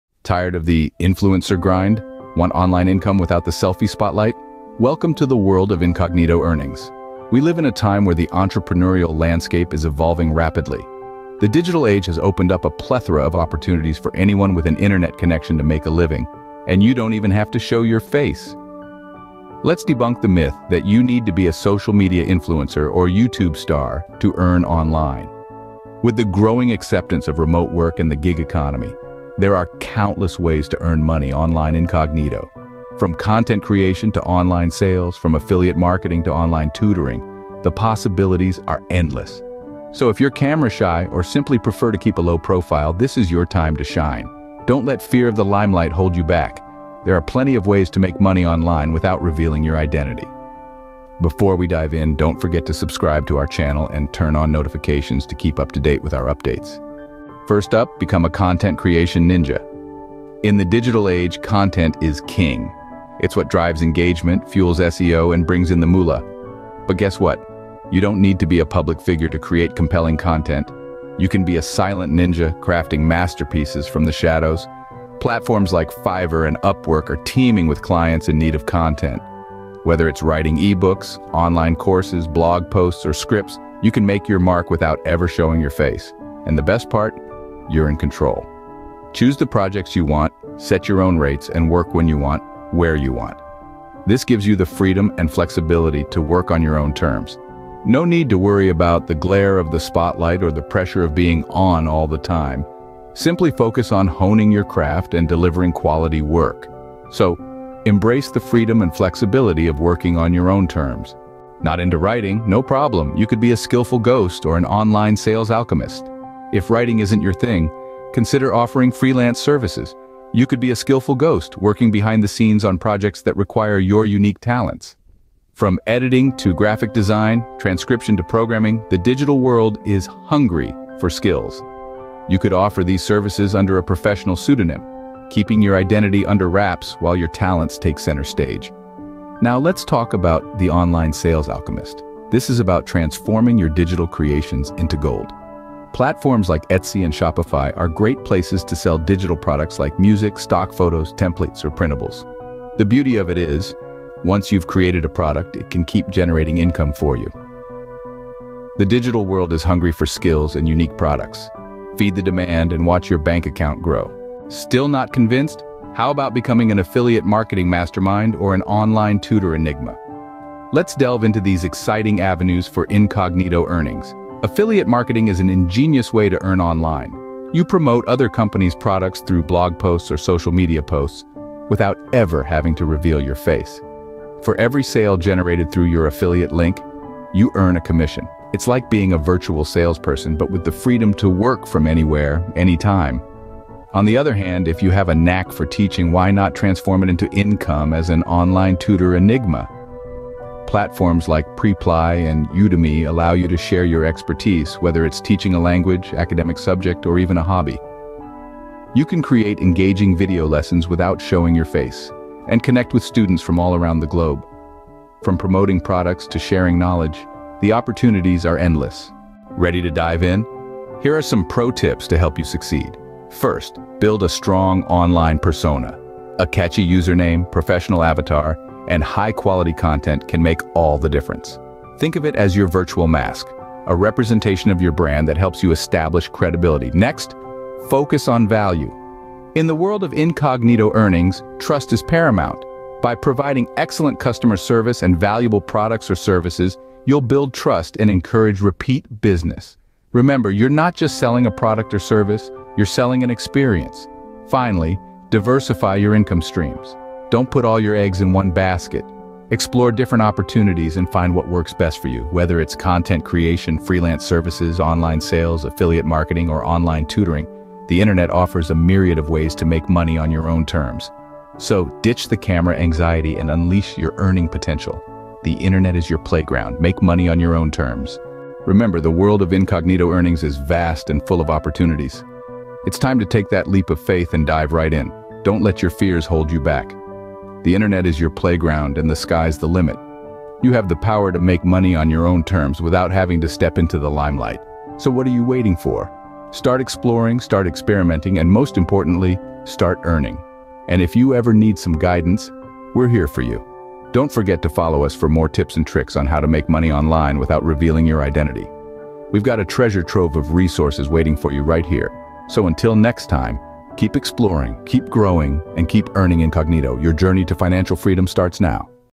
Reddit Stories | Secretive Strategies | 5 Ways to Earn Money Online
Buckle up "Tired of the influencer grind? Want online income without the selfie spotlight? Welcome to the world of incognito earnings!"
Fast-paced montage of people working remotely on laptops, hidden behind masks and sunglasses. Coins and dollar bills rain down.
OUTLINE:
00:00 The Entrepreneurial Evolution
00:57 Content Creation Ninja
01:51 Skillful Ghost & Online Sales Alchemist
02:44 Affiliate Marketing Mastermind & Online Tutor Enigma
03:39 Pro Tips for Incognito Earnings
04:38 Make the Internet Your Playground